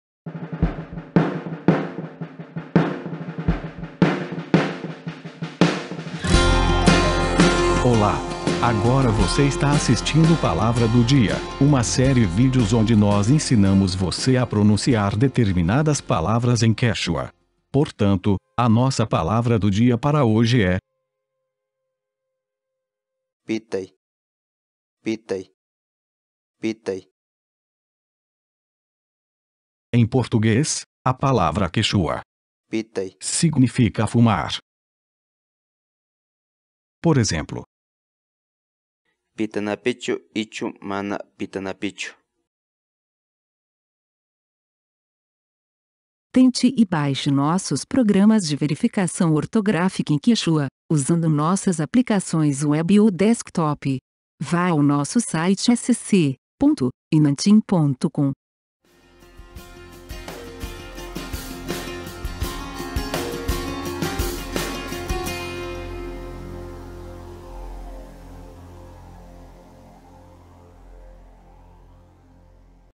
Como se pronuncia pitay ‘fumar’ (PT)
Social

Grupo de Pesquisa Hinantin: A entrada ‘pitay‘ faz parte da base para o desenvolvimento de um dicionário falado disponível livremente para a linguagem quéchua.